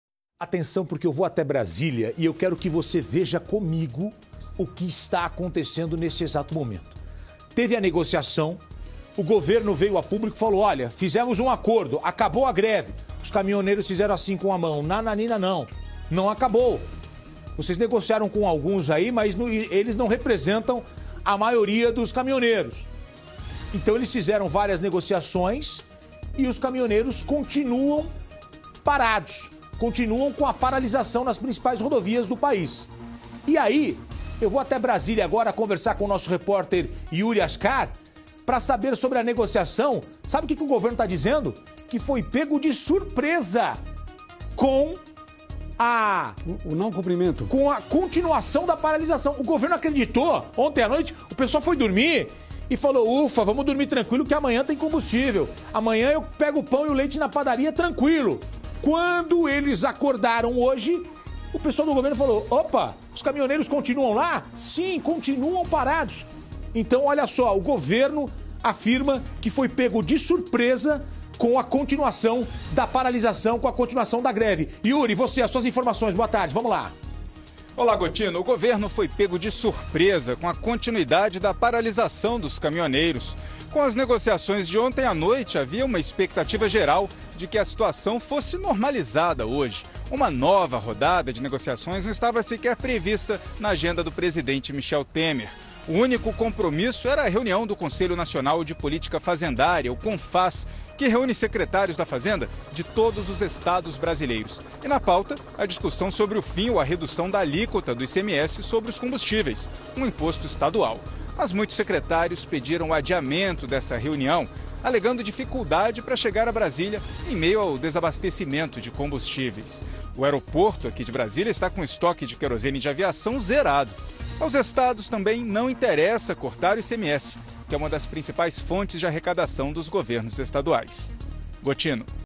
Governo afirma que foi pego de surpresa com a continuação da greve dos caminhoneiros
Mesmo após firmar acordo com o governo na noite de quinta-feira (24), os protestos dos caminhoneiros continuam nesta sexta-feira (25) pelas rodovias estaduais e federais em todas as unidades federativas do País.